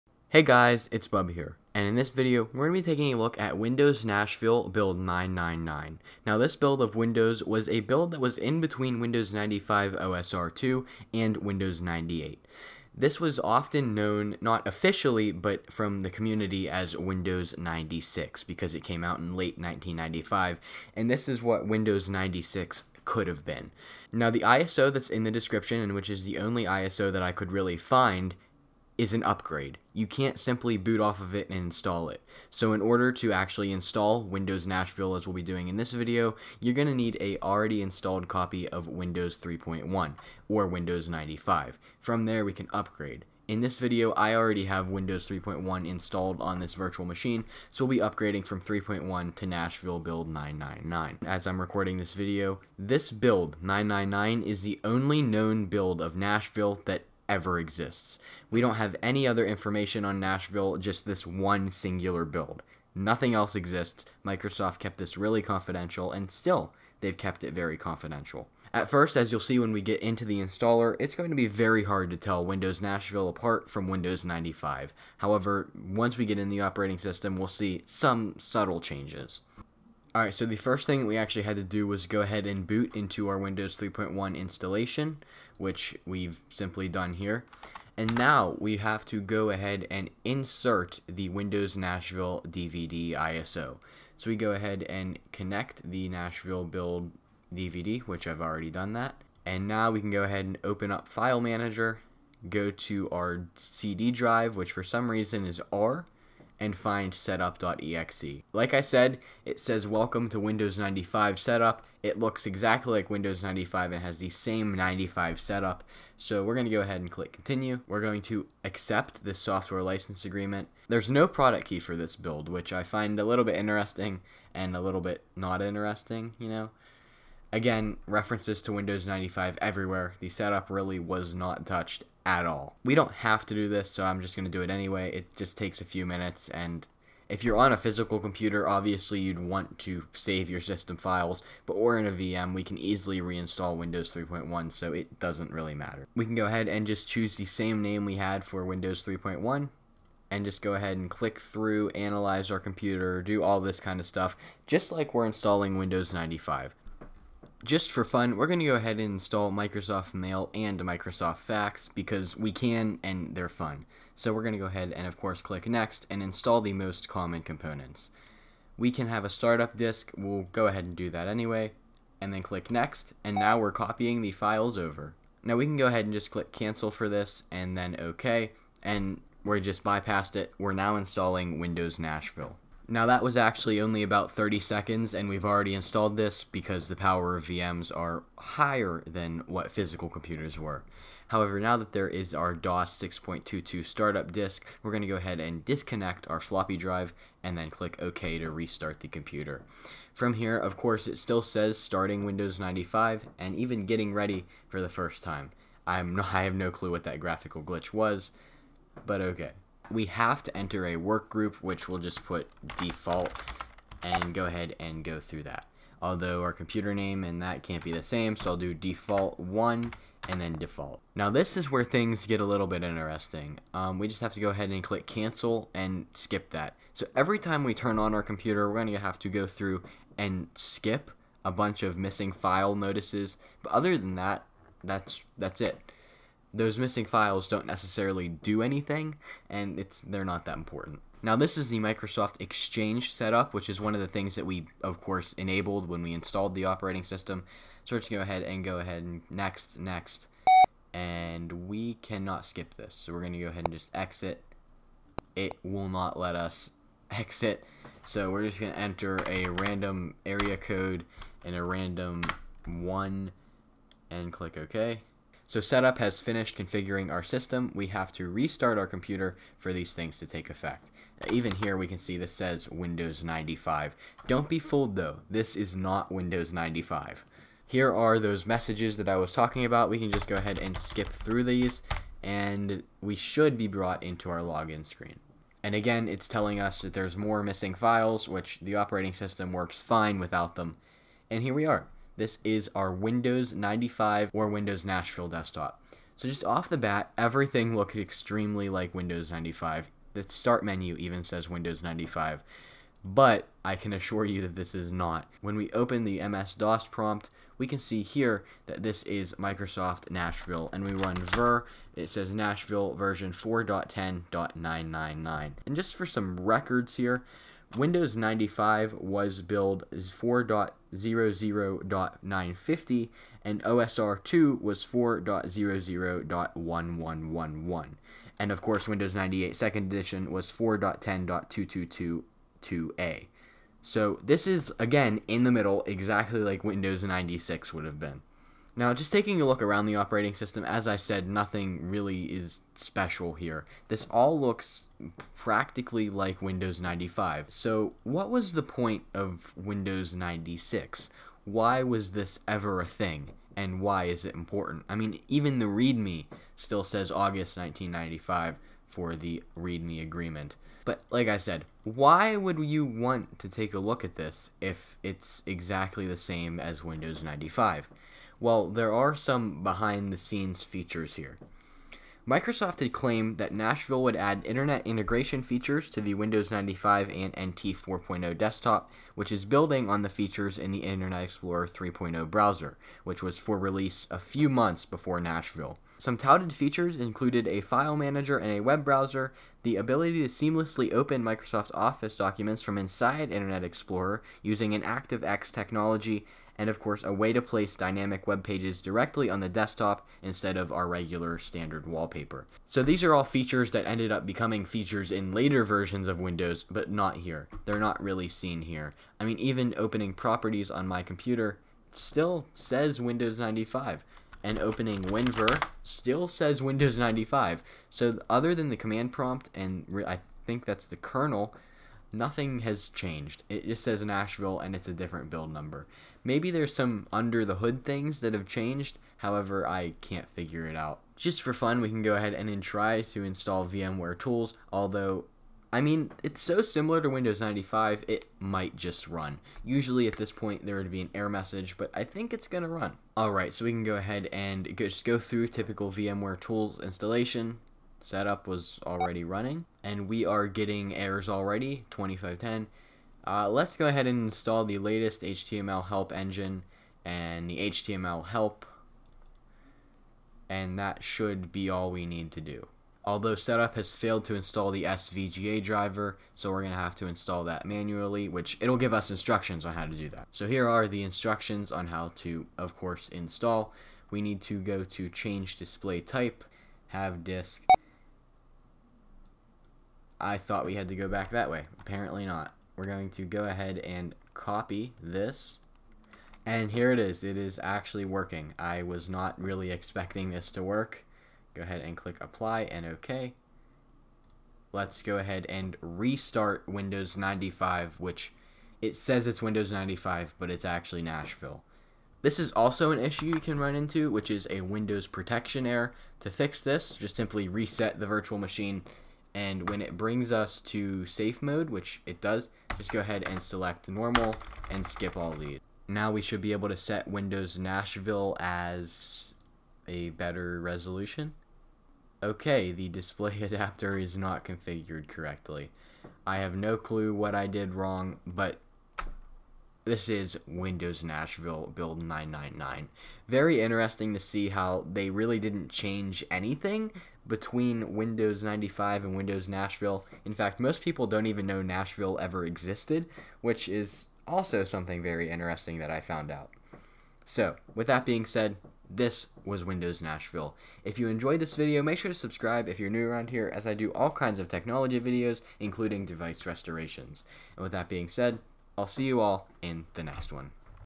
Windows Nashville Build 999 - Install and Overview
Windows Nashville was often referred to as Windows 96 - because it was created between Windows 95 OSR2 and Windows 98. As of 2021, Build 999 is the ONLY known build of Nashville. In this video, we'll be installing this OS, and seeing what's different between Windows 95 and this beta OS.